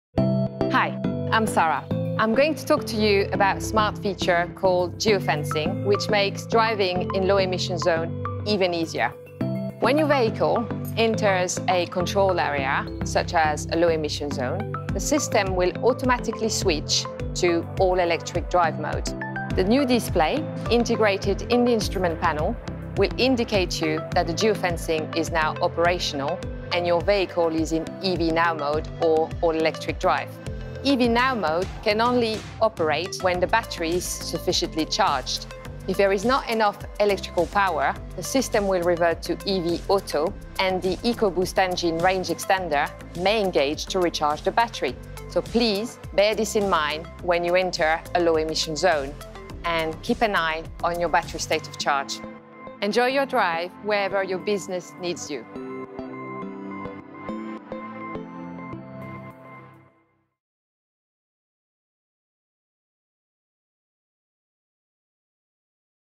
Tecnologia geofencing di Ford | Ford Italia
La funzione Geofencing di Ford: permette alla nuova generazione di veicoli commerciali Plug-In Hybrid di muoversi in modalità completamente elettrica nelle aree più sensibili.

La tecnologia mira a garantire che l’aria sia più pulita dove è maggiormente necessario, permettendo alla sua nuova generazione di veicoli commerciali Plug-In Hybrid di muoversi in modalità completamente elettrica nelle aree più sensibili.

Utilizzando i dati di localizzazione in tempo reale, i Transit Custom Plug-In Hybrid (PHEV) passano automaticamente alla modalità elettrica quando entrano in aree predefinite, a esempio, quelle a traffico limitato o a basse emissioni, contribuendo a mantenere l’aria più pulita nelle strade delle città.

Inoltre, la funzione Geofencing, non opera solo nei centri urbani: gli utenti possono anche creare “Aree verdi” personalizzate per utilizzare una guida a basse emissioni (modalità EV Now) nei pressi di luoghi come le scuole, i parchi giochi e depositi.

Quando il veicolo lascia una “zona a basse emissioni” può passare automaticamente alla modalità di guida più appropriata per completare la tappa successiva del viaggio, a esempio, coinvolgendo il motore a benzina EcoBoost come range extender, per generare energia elettrica ed estendere l’autonomia del veicolo.